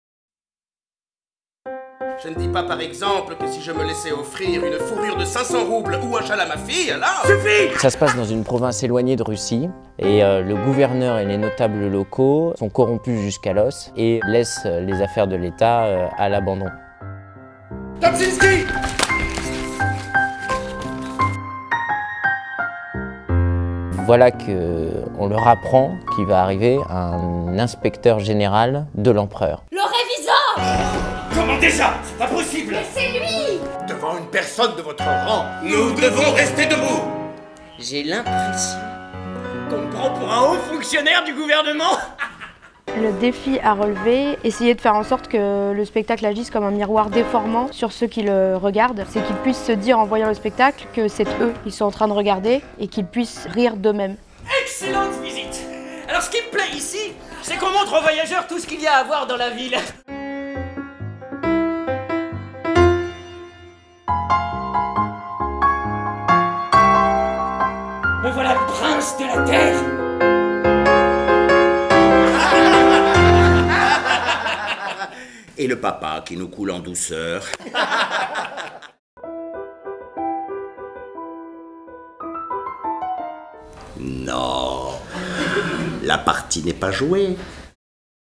Le Revizor
Découvrez toutes les vidéos des spectacles et loisirs de votre région

Revizor I Festival Off D'Avignon au Petit Louvre

Dans une province russe éloignée, un jeune aristocrate oisif est pris pour un inspecteur général du tsar en mission secrète.
Victimes de ce quiproquo, le Gouverneur et les notables locaux tentent de dissimuler leur gestion catastrophique de la ville et de corrompre cet inconnu.
Oscillant entre réalisme et fantastique, Gogol signe avec le Revizor son chef d’œuvre théâtral: une comédie jubilatoire sur l'imposture qui saisit les personnages dans un stupéfiant tourbillon d'humour et d'angoisse, jusqu'à l'effroi final...

Le Revizor pour le Festival Off d'Avignon au Petit Louvre